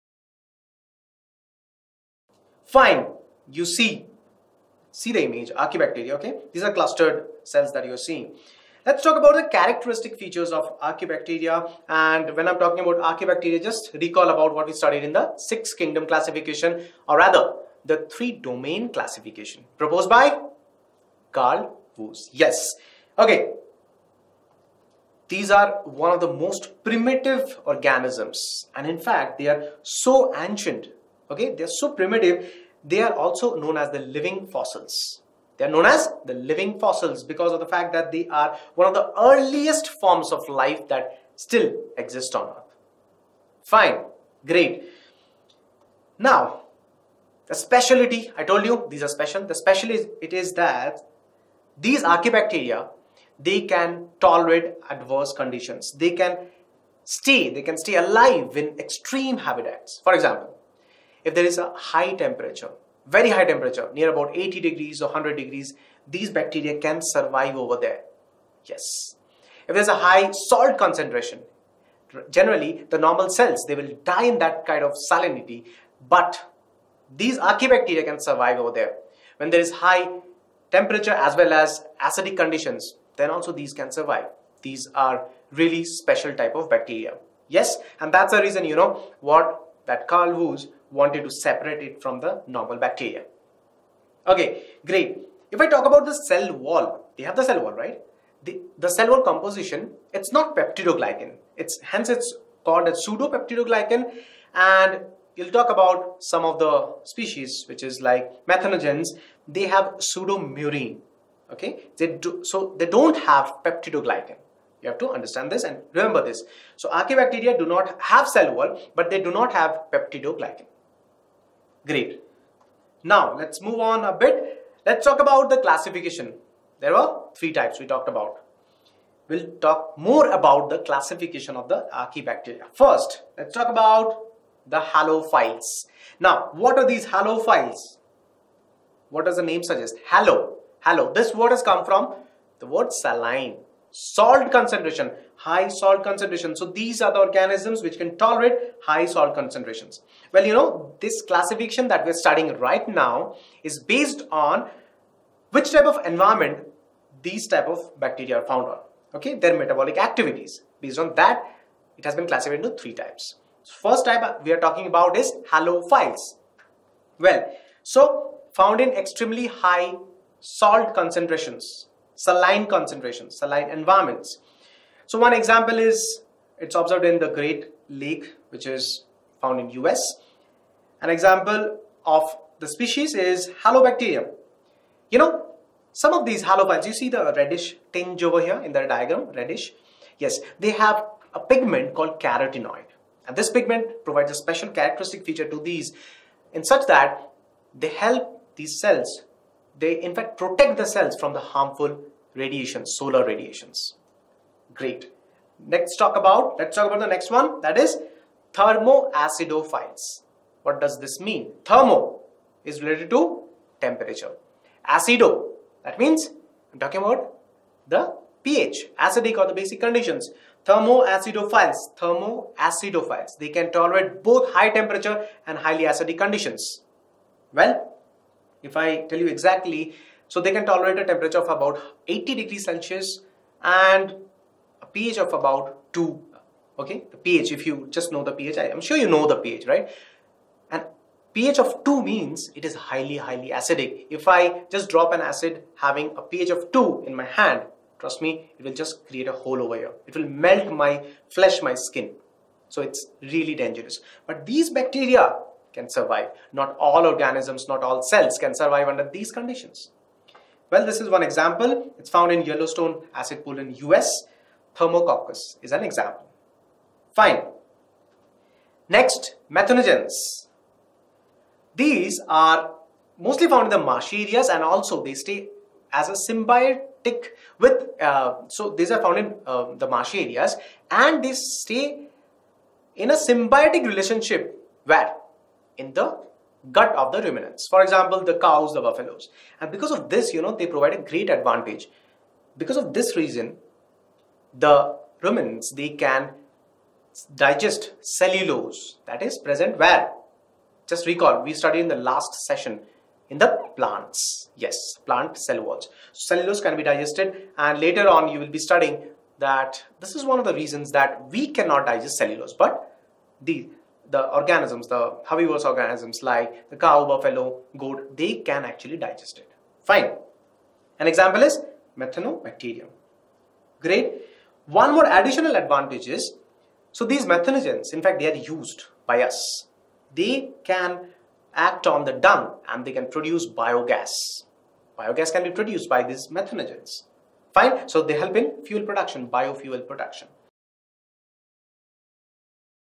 Archaebacteria | Biology | NEET | Concept of the day | Pushpendu Sir
Relevant for Class: 11th
Chapter: Biological Classification
Prerequisites: Five kingdom classification, Three domain classification, Kingdom Monera
This video from the chapter Biological Classification will help you revise the concepts of the living fossils-Archaebacteria and their characteristic features. We will study in detail the three types of Archaebacteria- Halophytes, Thermoacidophiles, and Methanogens- and their exciting features!